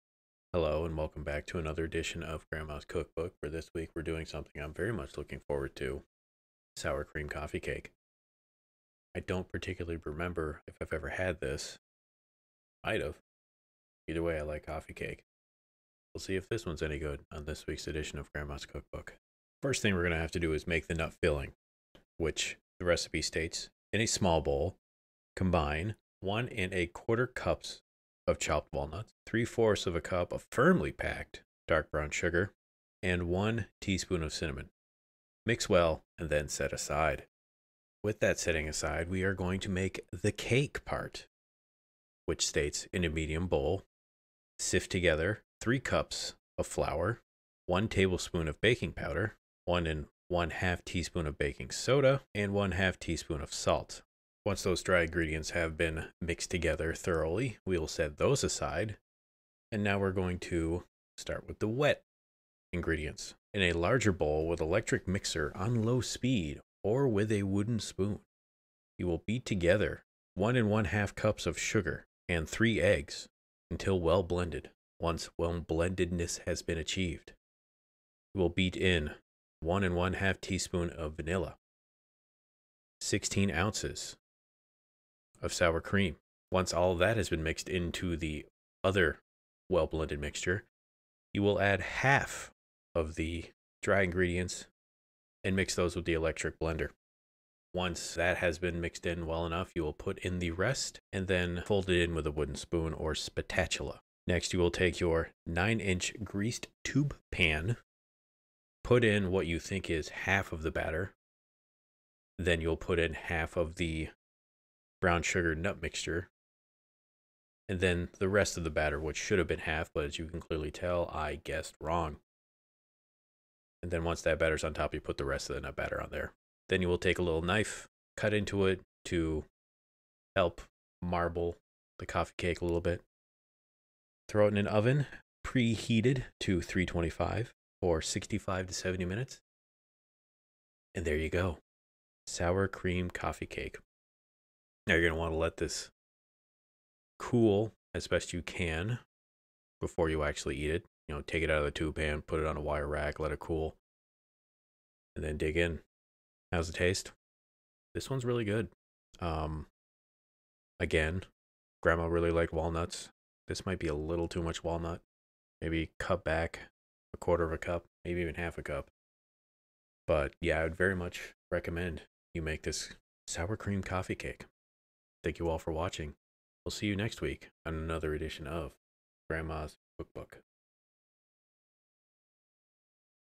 Grandma's Cookbook - Sour Cream Coffee Cake
Recipe as written:

Nut filling and topping
1 1/4 cup chopped walnuts
3/4 cup cup firmly packed, dark brown sugar
1 tsp. cinnamon

CAKE BATTER
3 cups all-purpose flour
1 Tbs. baking powder
1 1/2 tsp. baking soda
1/2 tsp. salt
1 1/2 cups sugar
3 eggs
1 16 ounce container sour cream (about 1 3/4 cups)
1 1/2 tsp. vanilla

Preheat oven to 325. Grease 9in tube pan. To prepare nut filling, in a small bowl, combine walnuts, brown sugar and cinnamon; mix well and set aside. To prepare cake, in a medium bowl; sift together flour, baking powder, baking soda and salt; set aside. In a large bowl, with electric mixer on a low speed or with a wooden spoon, beat sugar and eggs until well blended. Beat in sour cream and vanilla. Add half of the flour mixture, beating until smooth. Fold in remaining flour mixture.
Pour half of the cake batter into pan; sprinkle evenly with half of the nut mixture.
Spoon in remaining batter. Top with remaining nut mixture.
With a knife, cut into batter to marble in nut topping. Bake 65-70 minutes or until cake tester inserted in center comes out clean. Remove outer rim of the pan; allow to cool completely. Remove cake from tube onto serving plate.